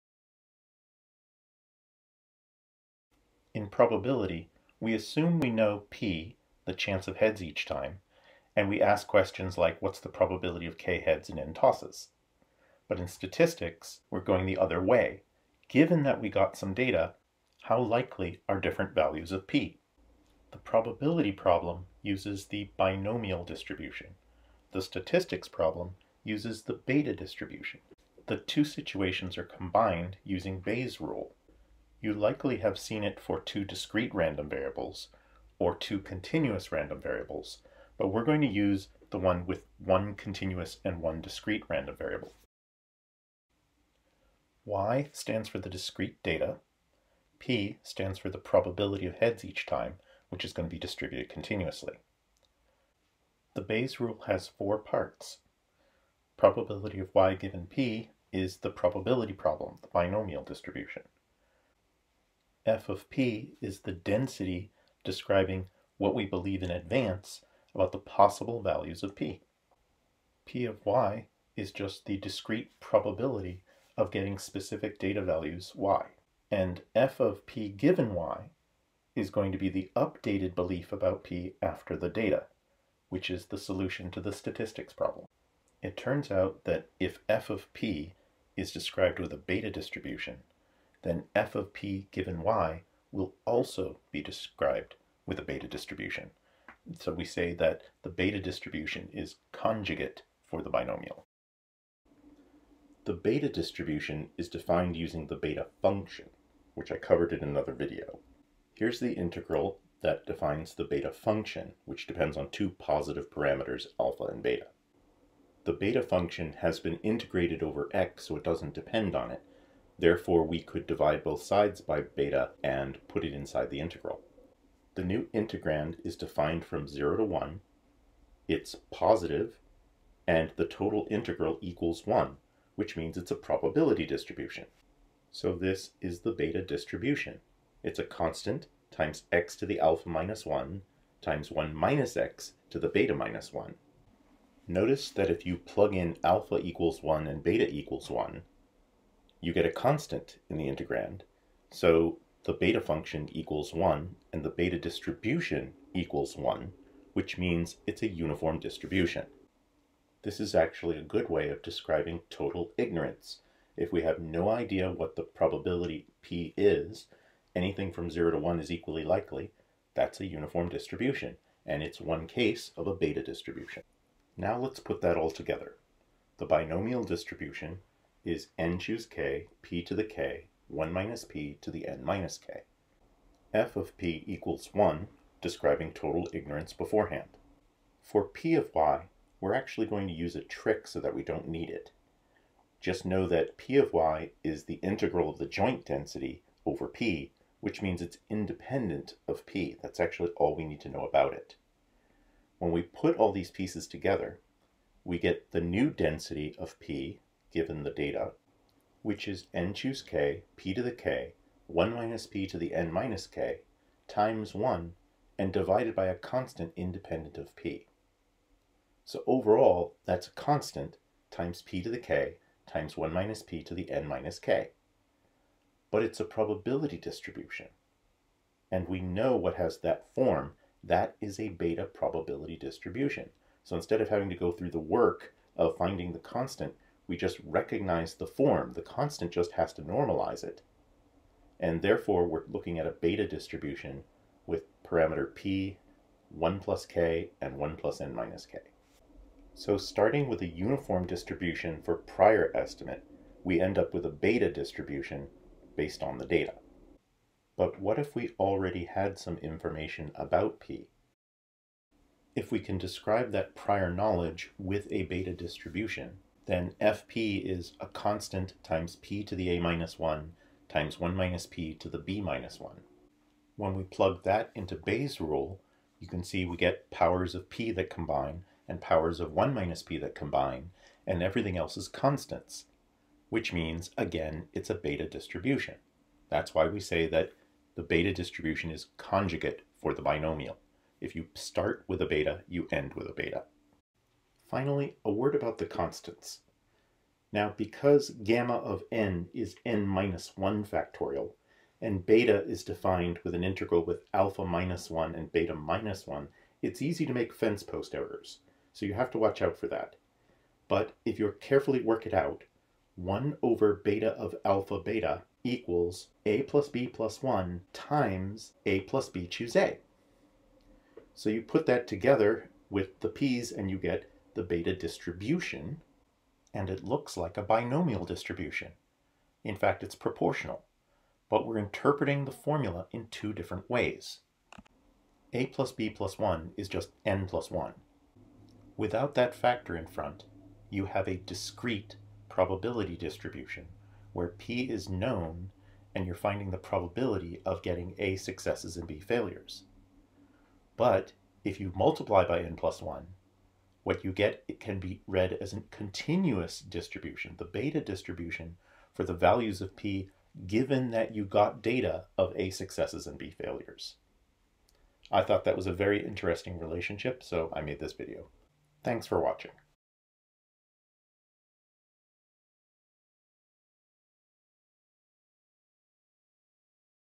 How Beta is Conjugate to Binomial
A derivation of the relationship between prior beta distribution, binomial data, and an updated beta distribution.